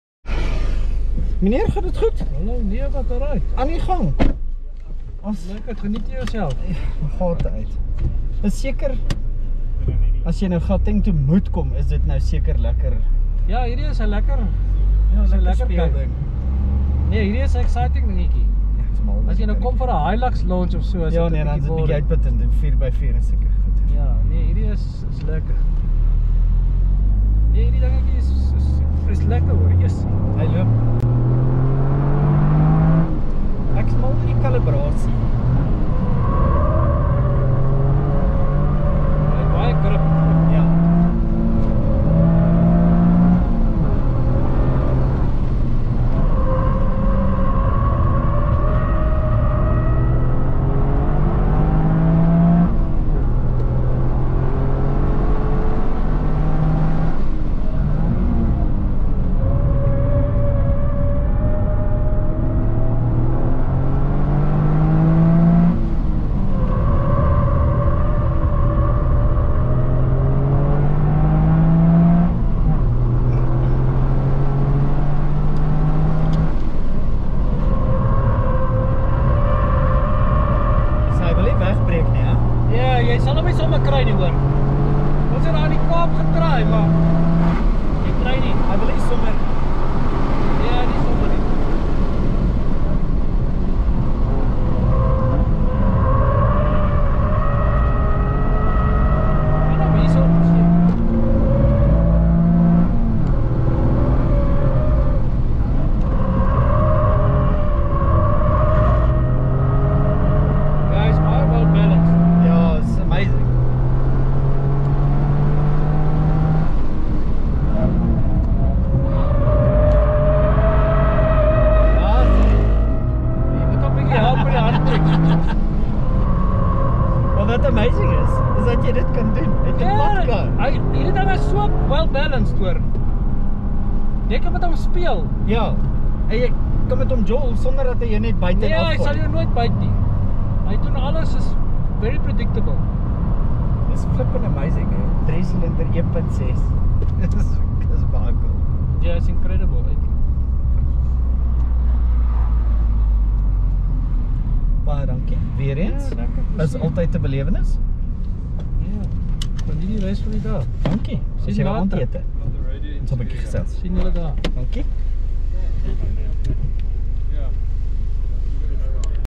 Toyota GR Yaris
Maroela Media se Brendon Staniforth ry saam met Giniel de Villiers.